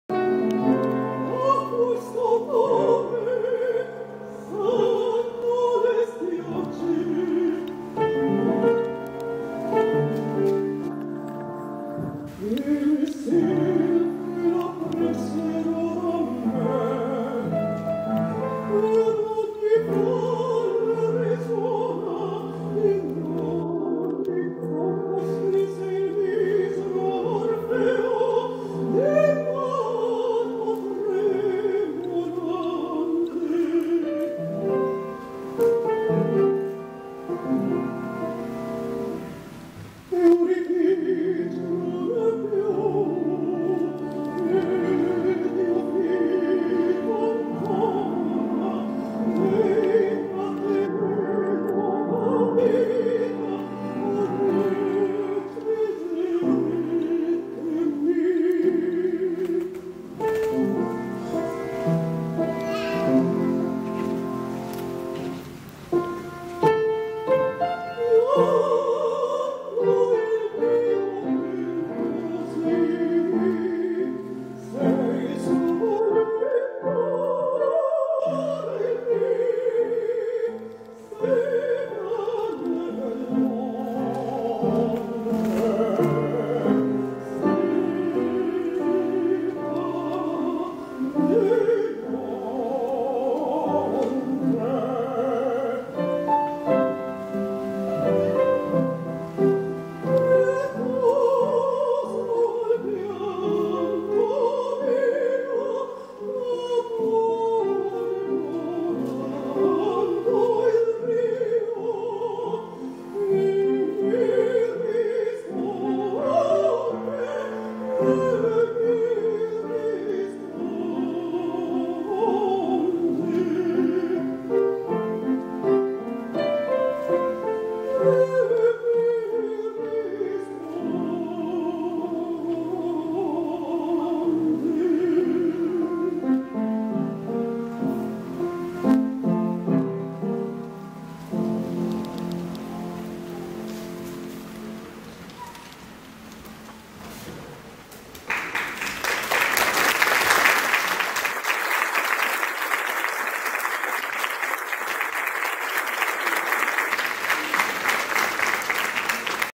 Ópera Explícita Segunda Edición, Primera Sesión * SAM 0144
Ópera Explícita Segunda Edición, Primera Sesión.
La Reforma: Movimiento Inaugural del Clasicismo.

Jueves, 14 Abril 2016
Sala Federico Campbell,
Centro Cultural Tijuana!